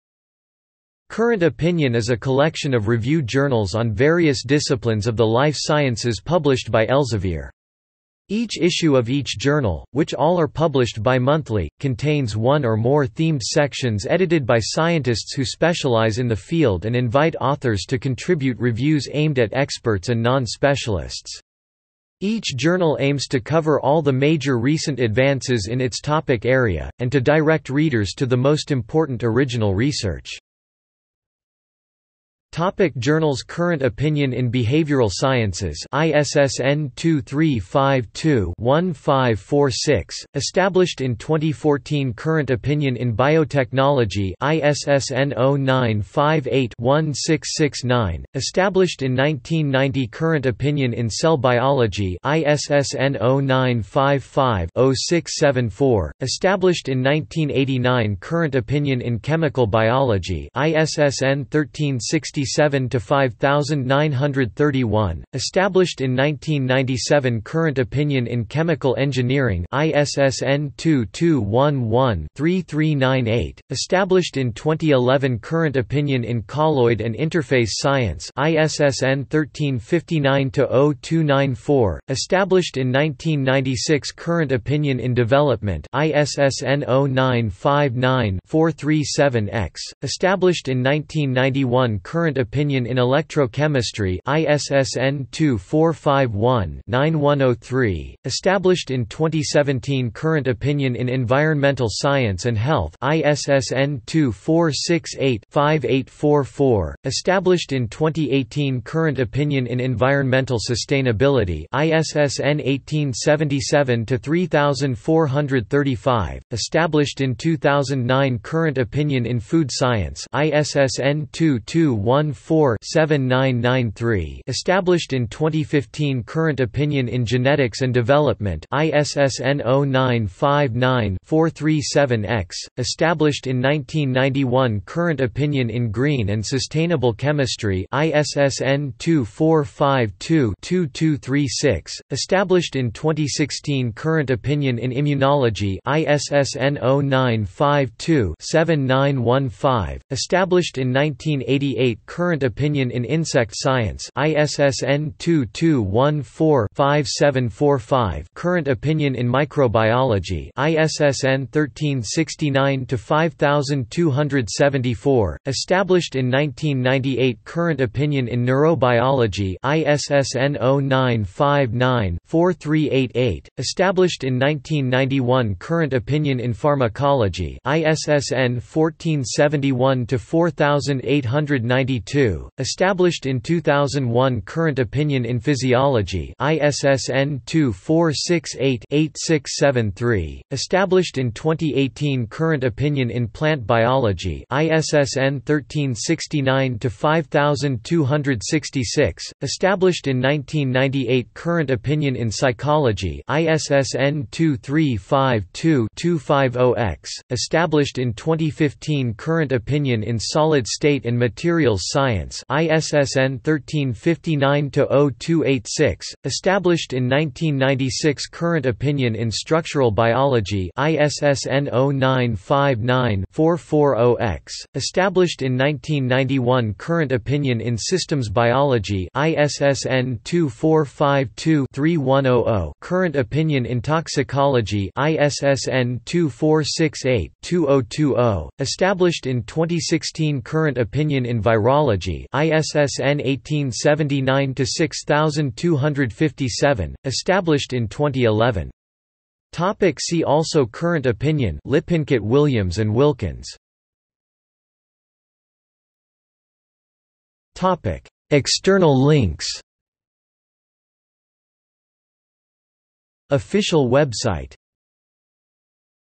Current Opinion in Structural Biology | Wikipedia audio article
00:00:32 Journals
00:04:09 See also
00:04:13 External links

- Socrates

SUMMARY
Current Opinion is a collection of review journals on various disciplines of the life sciences published by Elsevier. Each issue of each journal, which all are published bimonthly, contains one or more themed sections edited by scientists who specialise in the field and invite authors to contribute reviews aimed at experts and non-specialists. Each journal aims to cover all the major recent advances in its topic area, and to direct readers to the most important original research.